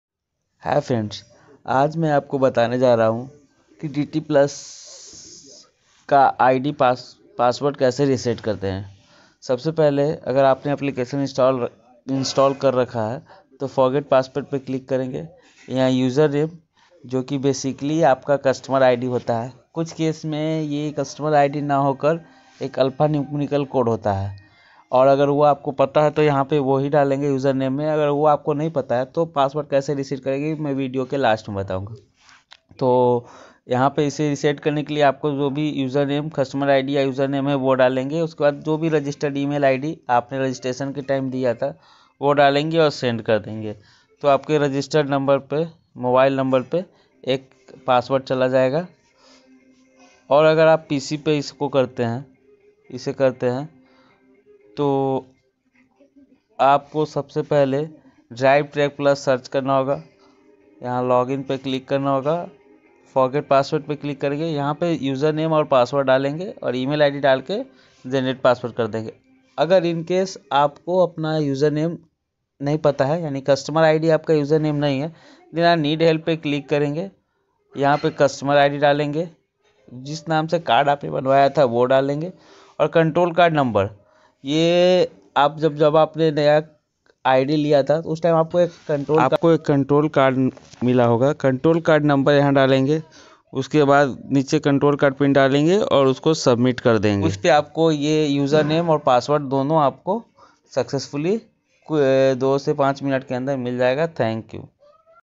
HPCL Drive Track Plus How to Reset Password
In this Video You Get knowledge about hoe to reset password and also know how to reset password with the help of control Card No.